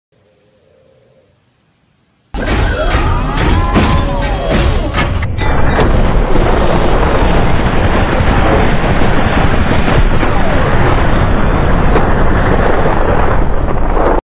COD: Black Ops 2 - Claw (SLOPRO)
First I filmed with the Action Movie FX app. Then I went into the SloPro app and put that video into my gallery. With some editing this is what I made. 😜 Happy Filming